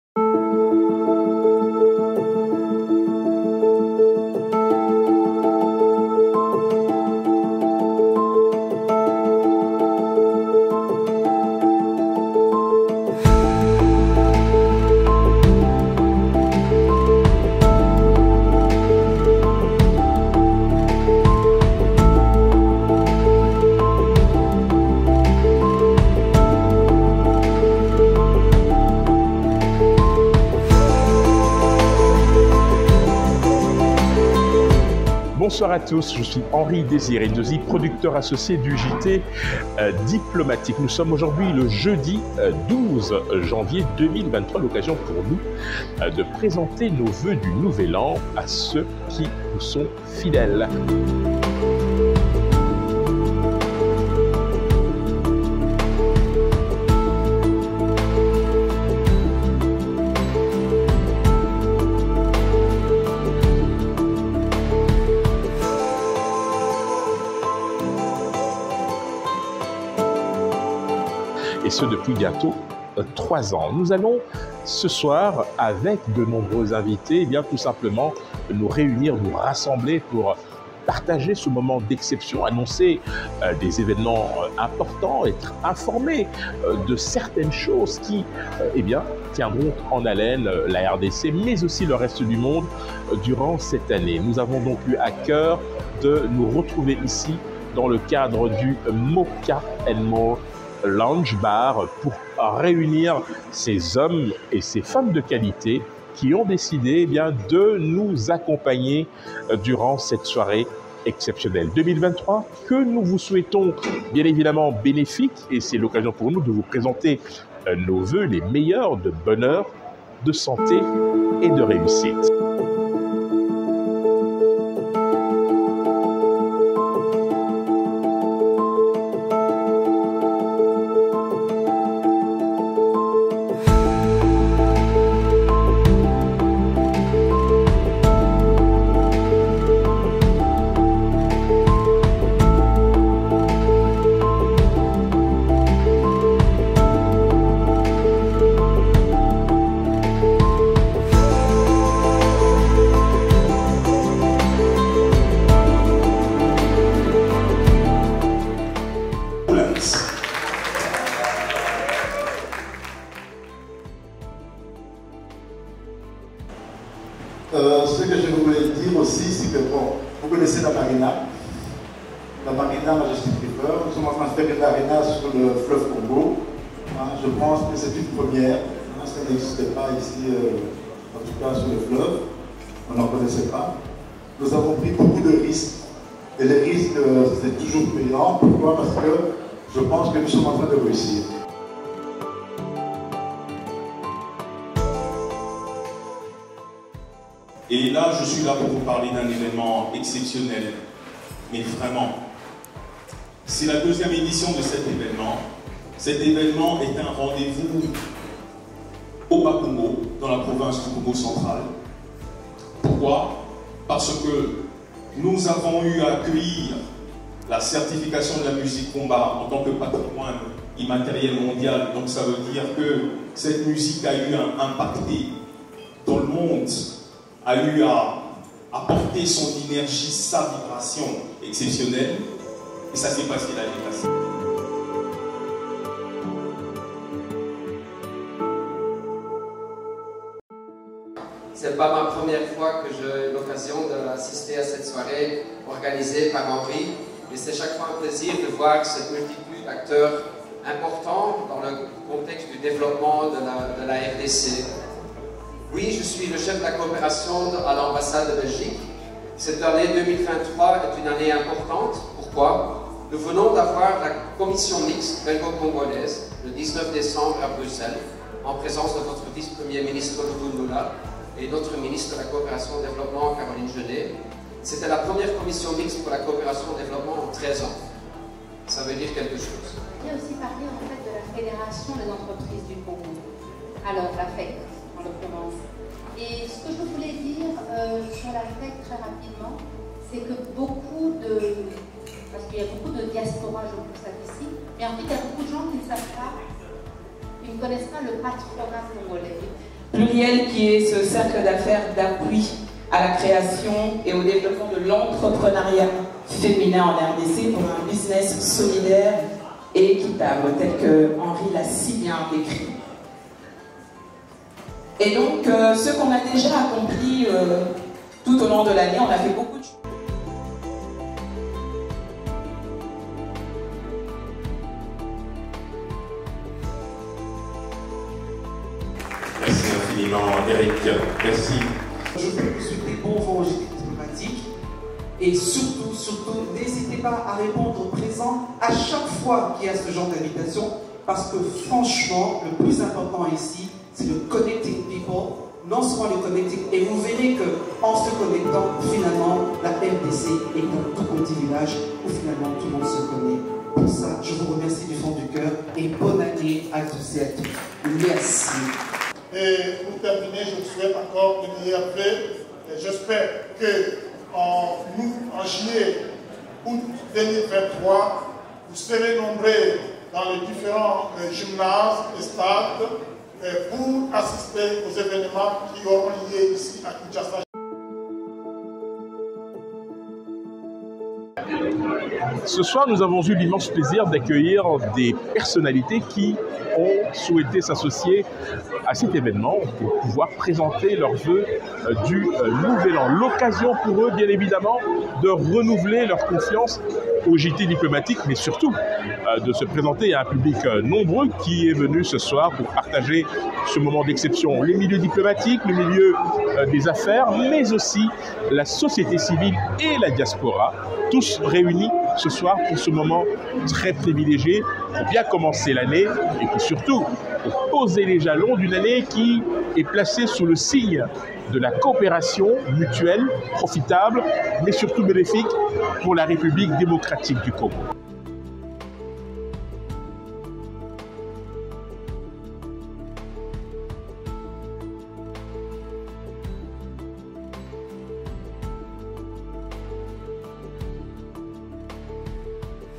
L'Officiel du JTD : Les Voeux 2023
Kinshasa, jeudi 12 janvier 2023 : Présentation des voeux du nouvel an du JT Diplomatique dans le cadre exclusif du Moka & More Lounge Bar.